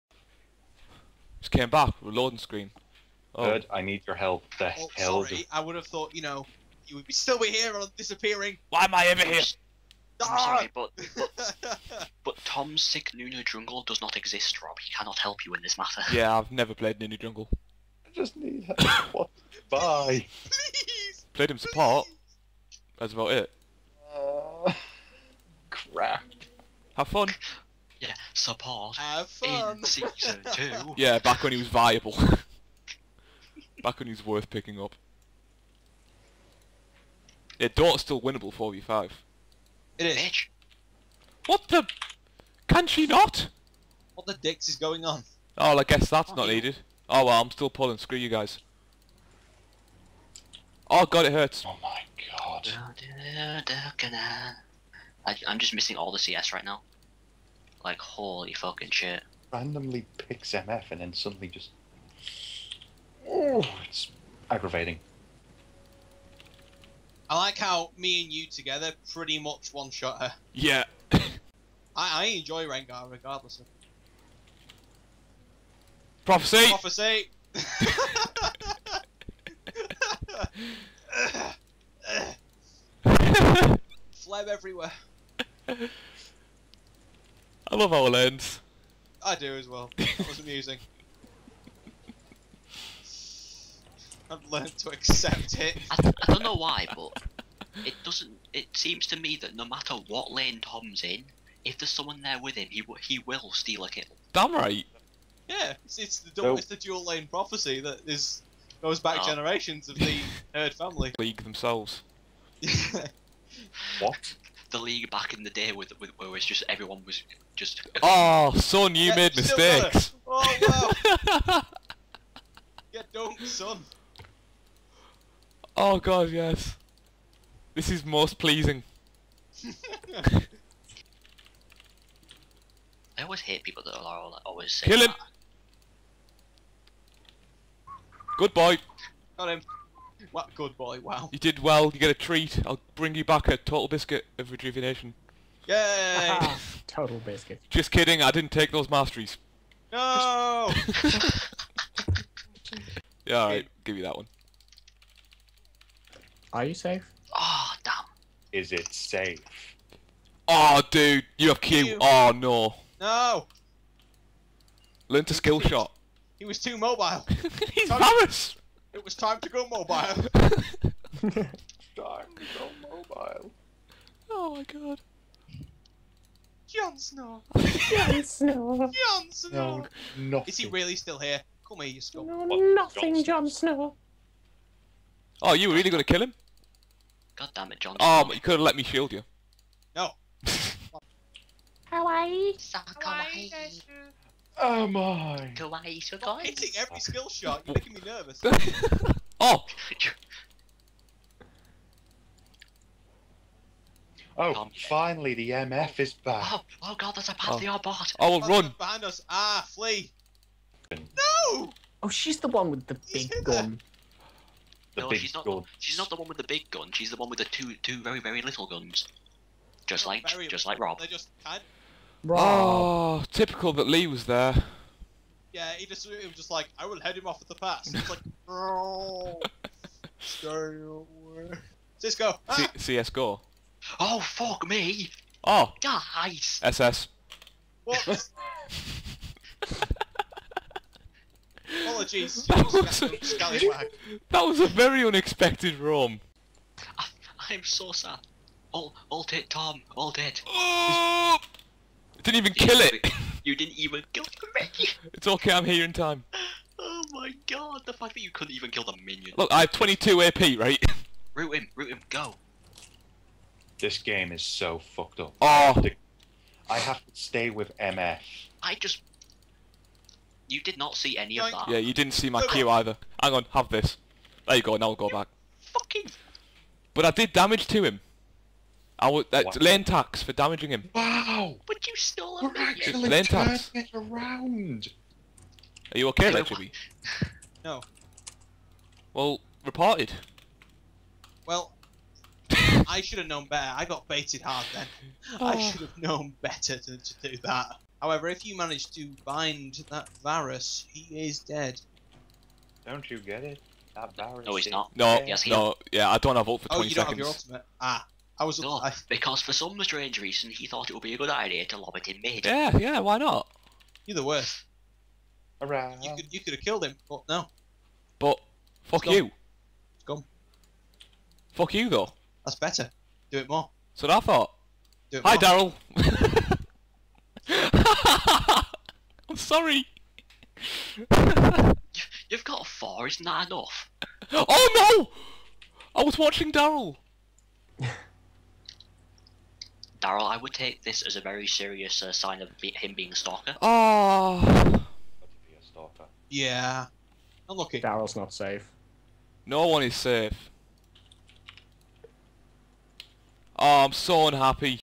THE PROPHECY OF BOT LANE
Me and my friend Chibby believe in the prophecy that every time we lane together I get a kill before him, it wins us lanes.